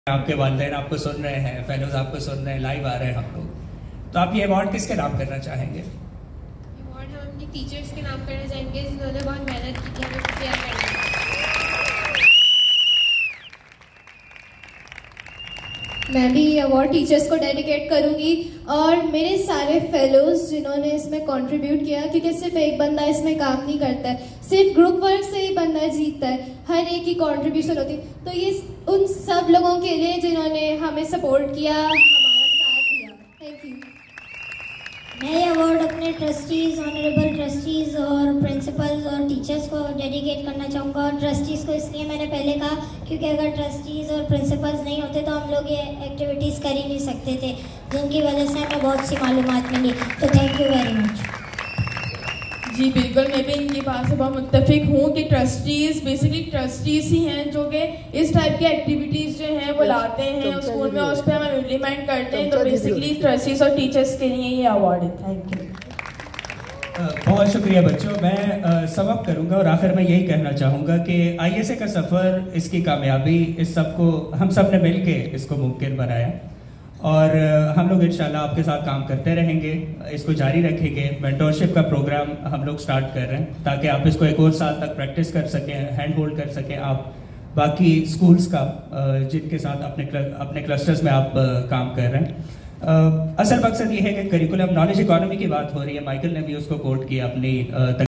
Shamsi School | international school award | ISA | public speaking | confidence building
shamsi school won 7 international school awards for the year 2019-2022 the ceremony held at pearl continental hotel Karachi.